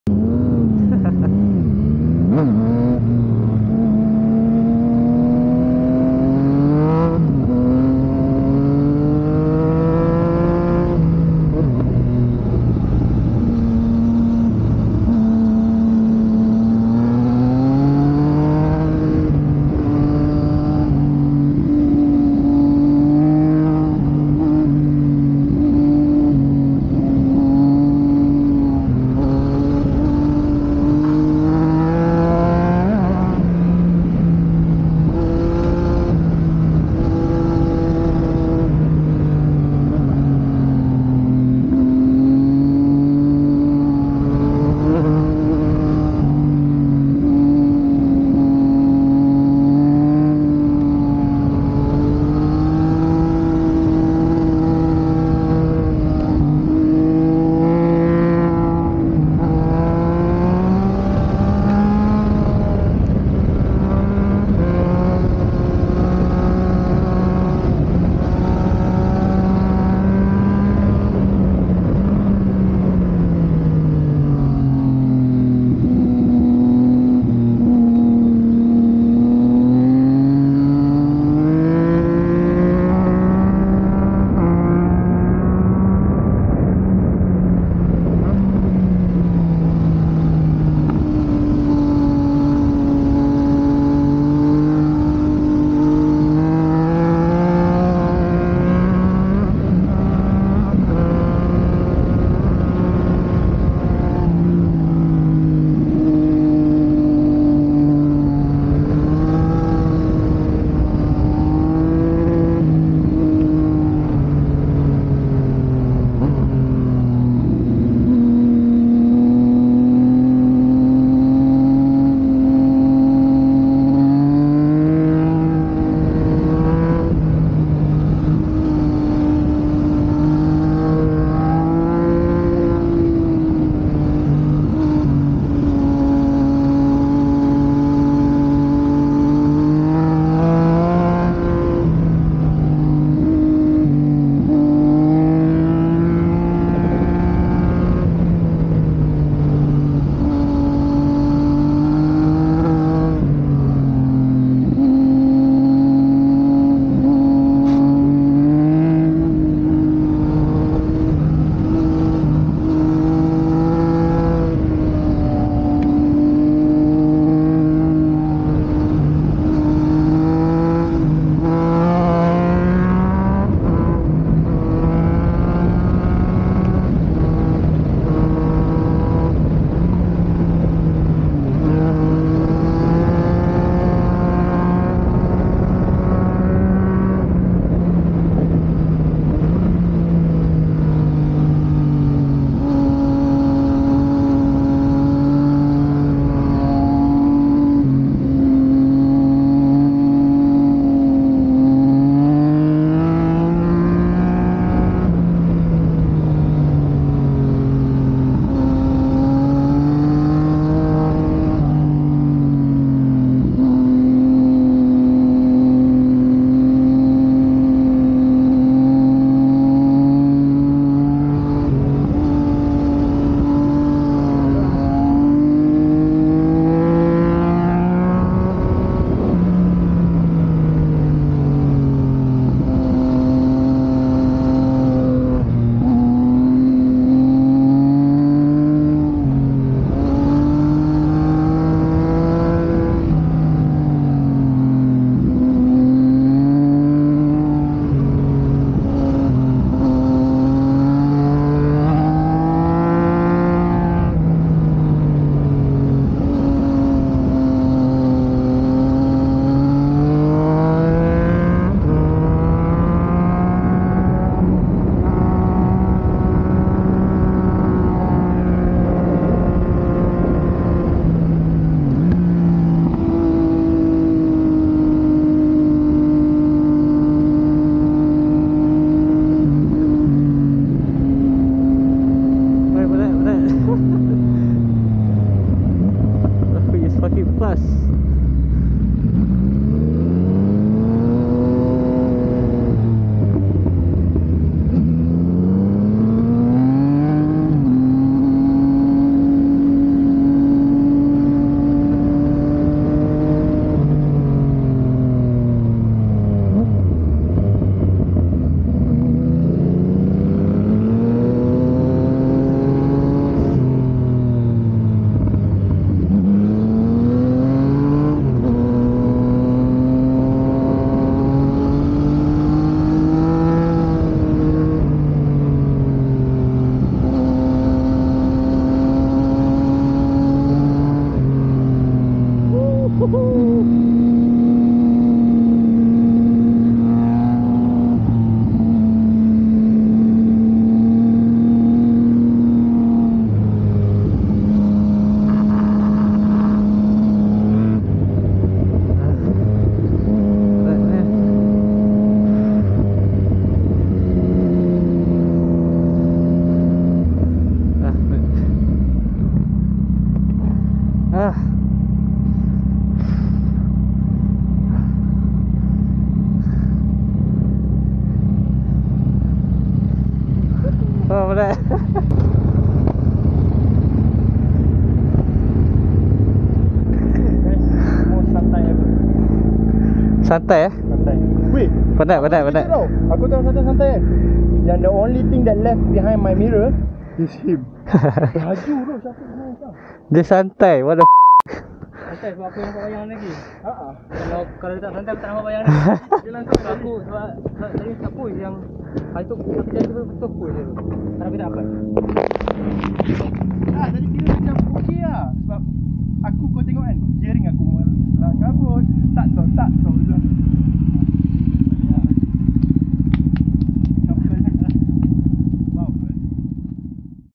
BTIC "Santai" Run ! | MT09 , DUKE 890 | Pure Sound [4K]
Rafiq's collab channel : @Hitman4President
Hairy's channel : @HairyIdhamMotovlog
Luqman's channel : @luqmanhakim8059